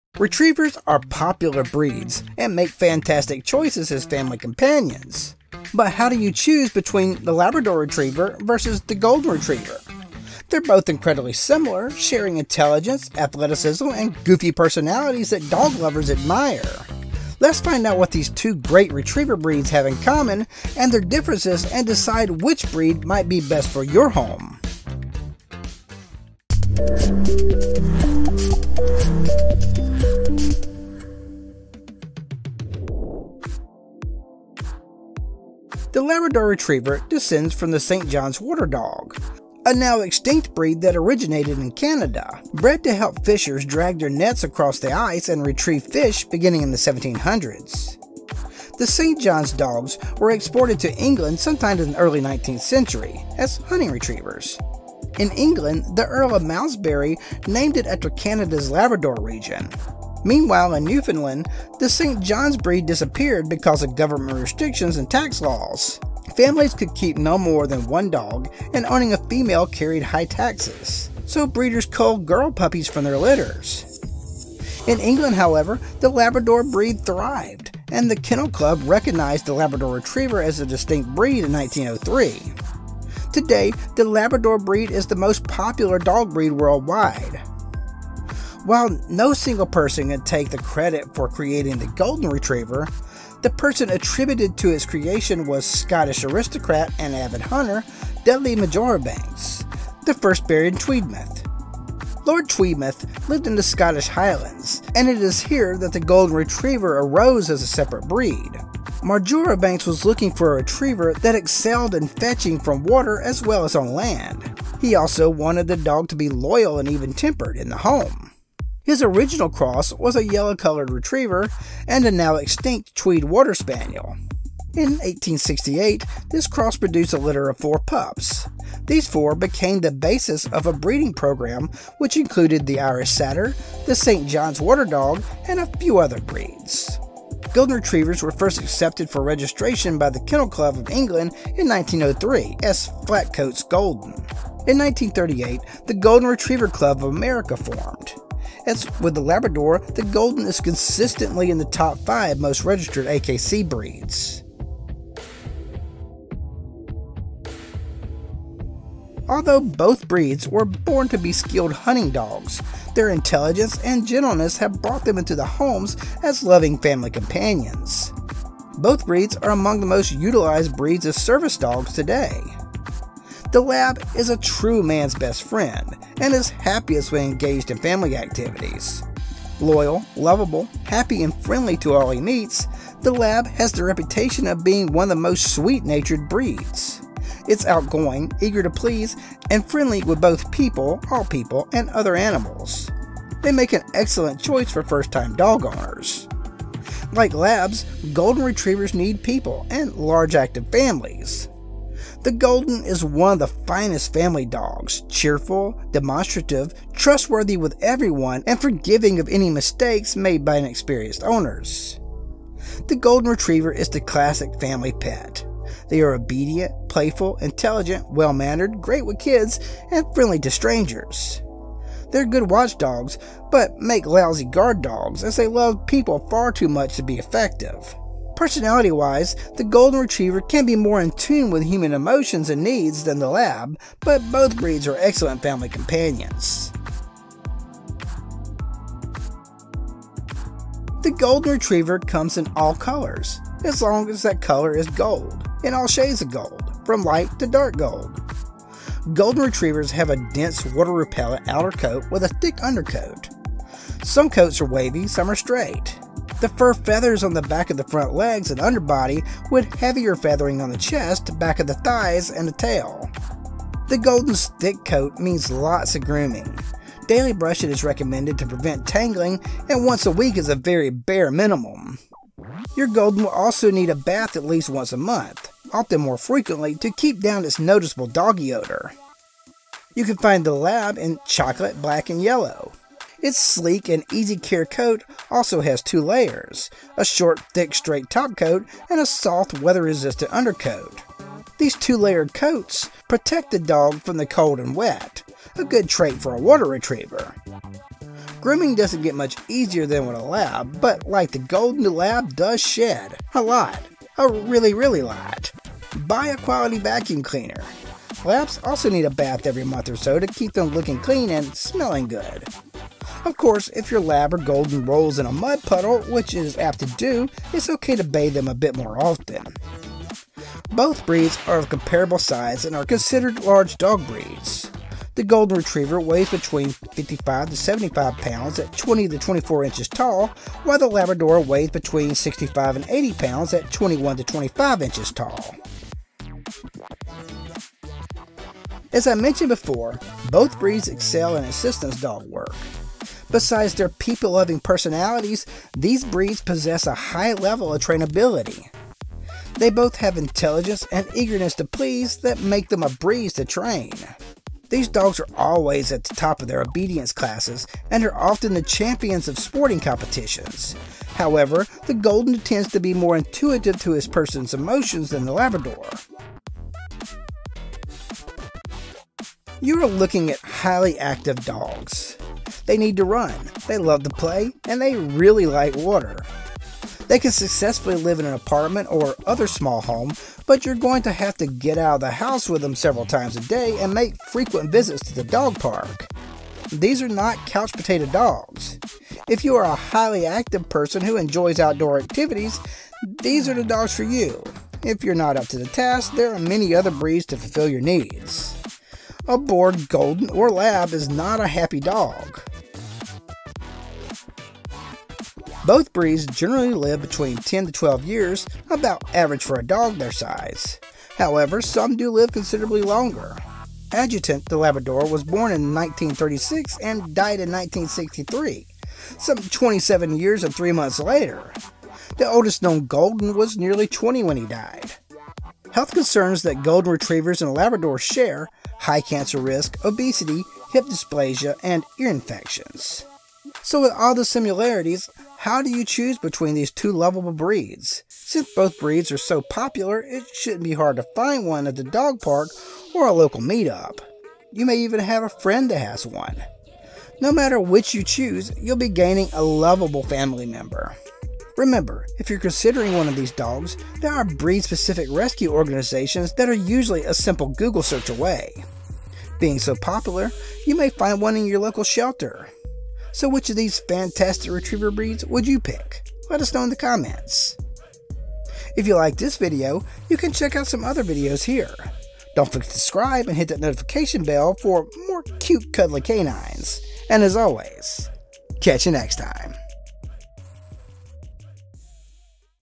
Labrador Retriever vs. Golden Retriever: Which is Better? Dog vs Dog
Labrador Retriever vs. Golden Retriever: Which is Better? Dog vs Dog Are you having trouble choosing between a Labrador Retriever and a Golden Retriever? In this video, we'll explore the history, personality, size and appearance, trainability, activity levels, health and lifespan of these two popular breeds to help you decide which one is right for you. We'll also give you tips on where to find these lovable dogs, including breed-specific rescue organizations and local shelters.

0:00 Introduction to Labrador Retriever vs Golden Retriever
0:30 History of the Labrador Retriever
1:28 History of the Golden Retriever
2:28 Personality of the Labrador Retriever and the Golden Retriever
3:56 Size and Appearance of the Labrador Retriever and the Golden Retriever
4:39 Grooming needs of the Labrador Retriever and the Golden Retriever
5:35 Trainability of the Labrador Retriever and the Golden Retriever
6:09 Activity levels of the Labrador Retriever and the Golden Retriever
6:45 Health and Lifespan of the Labrador Retriever and the Golden Retriever
7:18 How to choose between Labrador Retriever vs. Golden Retriever